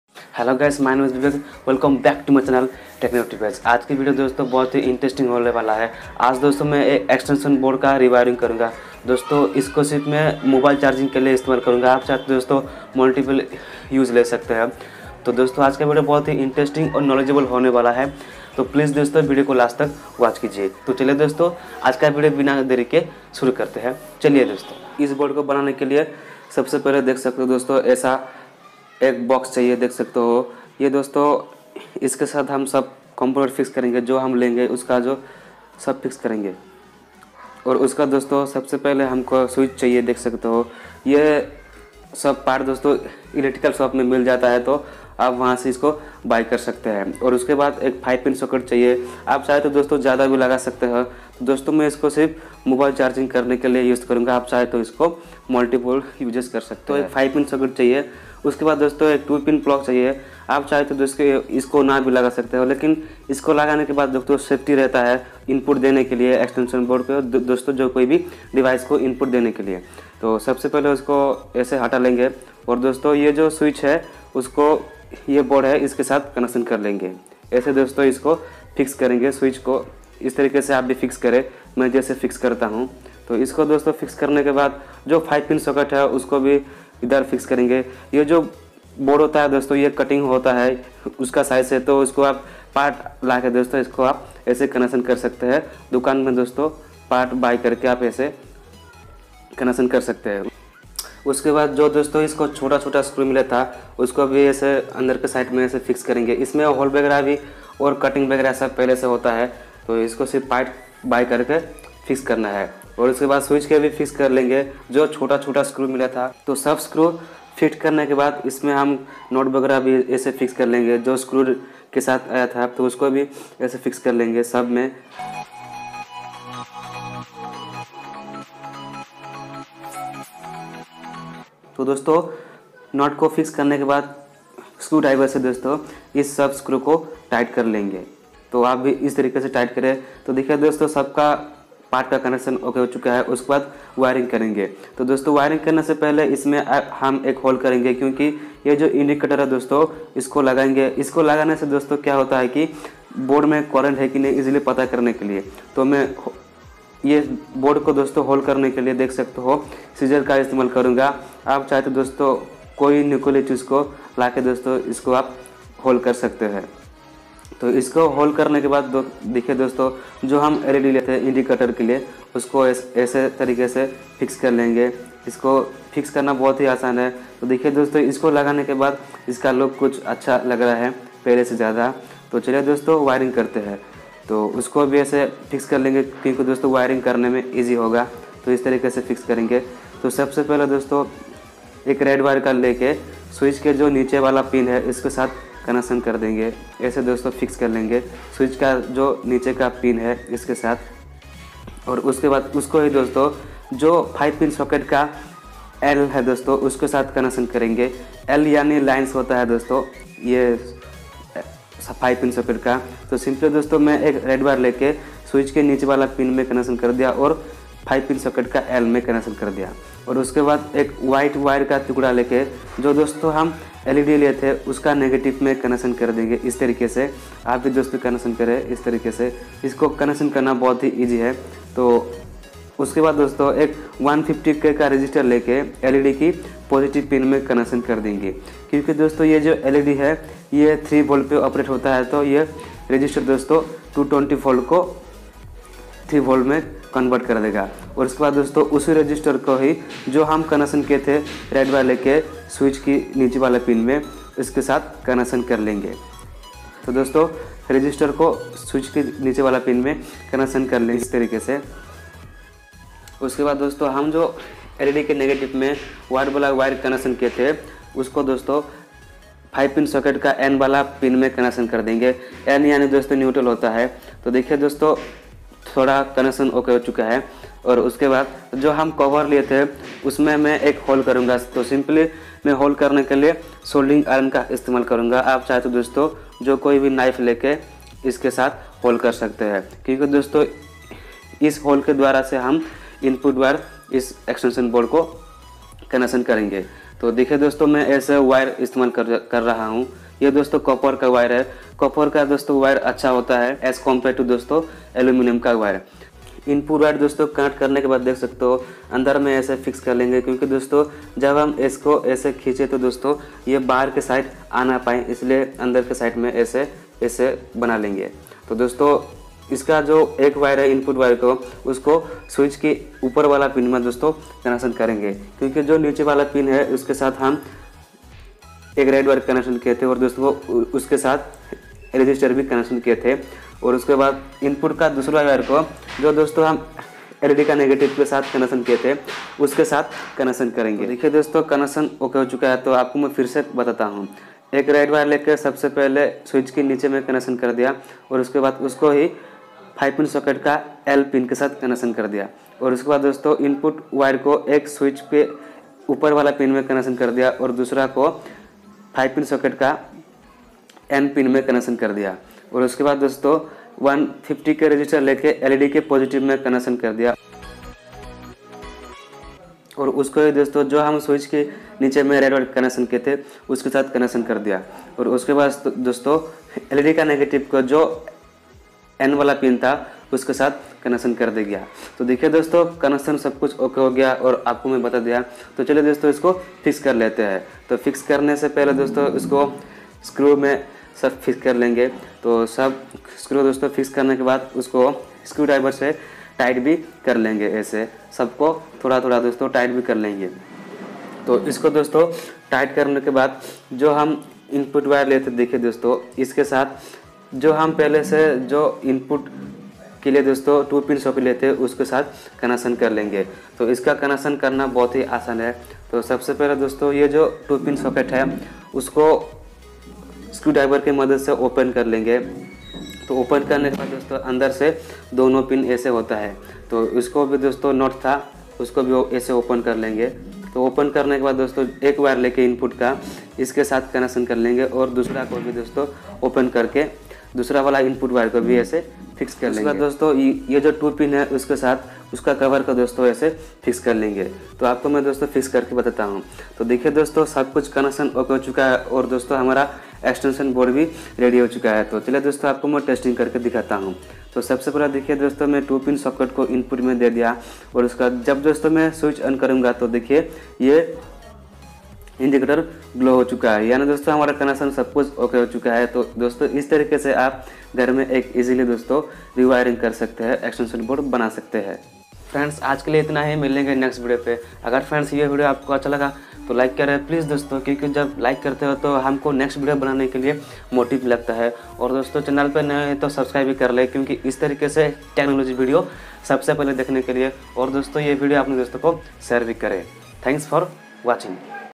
How to make extension board || Extension board कैसे करें || full details || 100% working
Hello dosto !
       aj main ek extension board Banya es ko banana bahat hi asan hai yeh dosto 100% working karta hai . es ko use karke app multiple gain paa sakte ho .

components uses :

1. Extension board cover
2. switch
3. 5pin socket
4. 2pin plug
5. led
6. 150k resistor

“Techno TVS"
from - Rayagada Odisha (India)